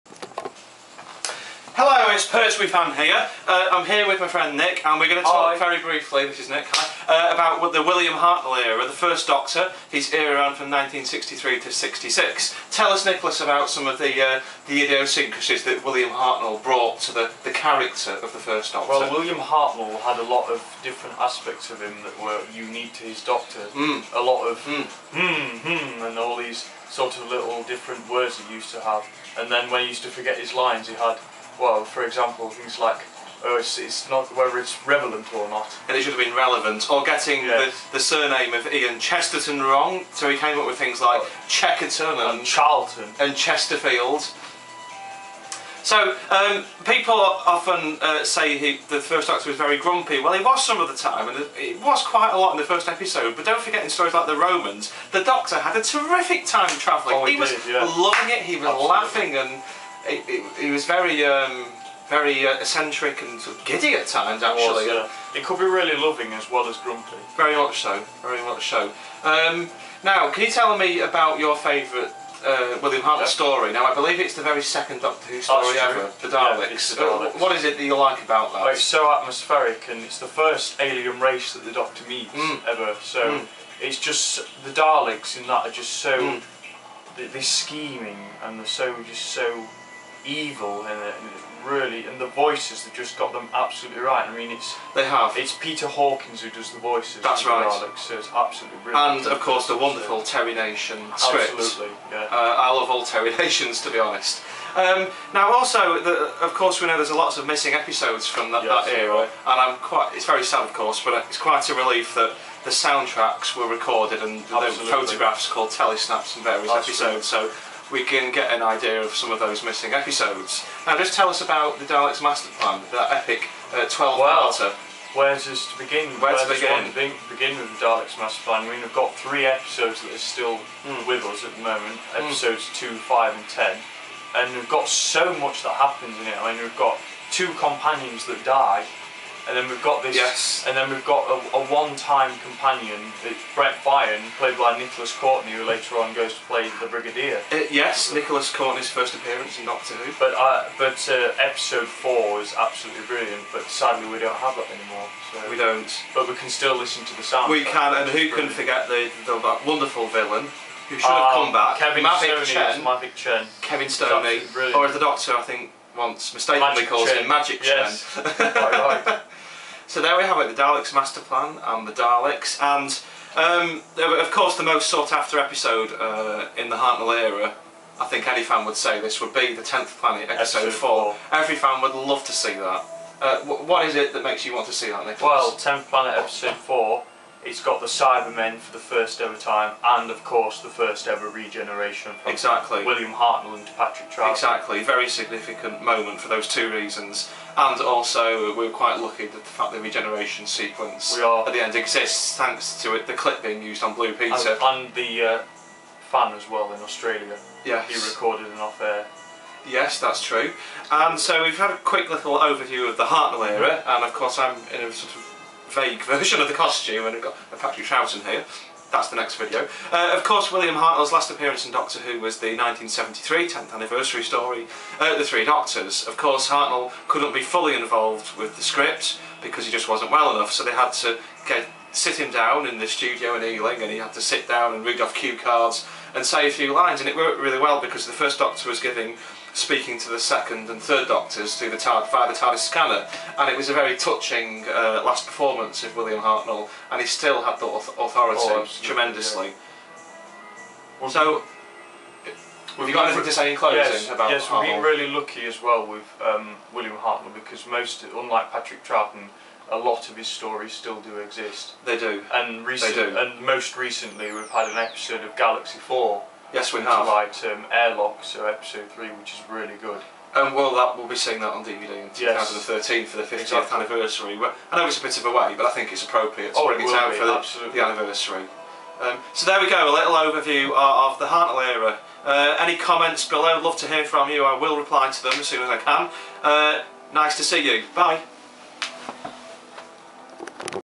William Hartnell era - overview
A short overview of the William Hartnell era, 1963-66 with my friend Nick. William's last appearance was of course in the 1973 story, The Three Doctors.